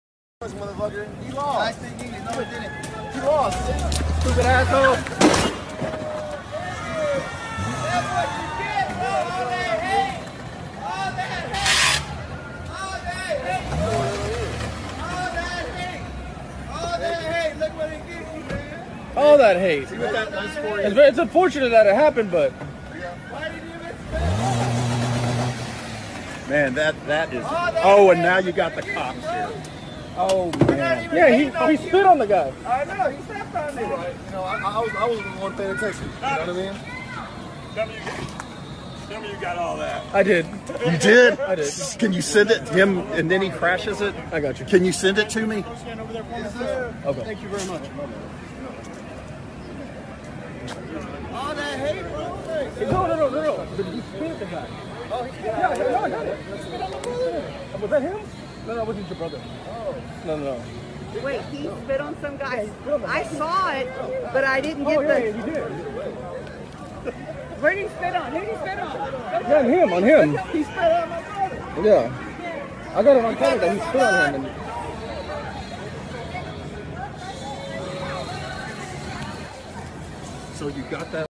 La Escupidota Nadie Se Las Quita, VALIO LA PENA
editan el video para que no escuches las consignas e insultos racistas de estos